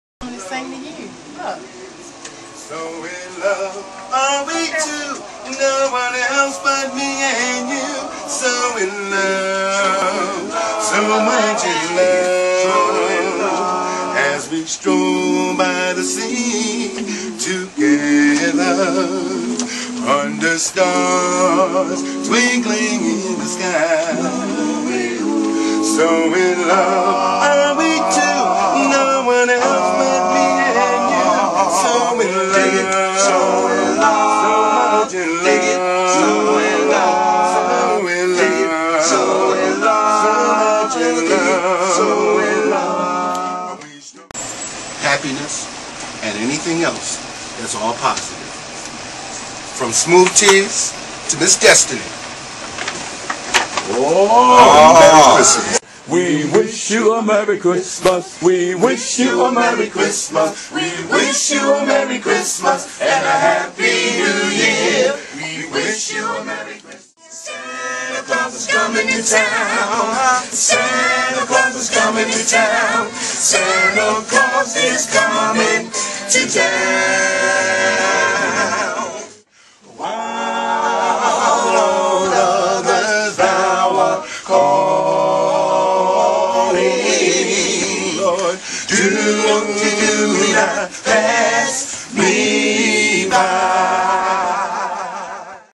Smooth Teaze Holiday Visit - Kennedy Krieger Institute
A capella "Doo-Wop" vocal group, Smooth Teaze, made a special holiday visit to Kennedy Krieger Institute. They made a patient's dream come true with a live performance and a Wii gaming system. Thank you to the Smooth Teaze! Enjoy a few highlights from their surprise visit.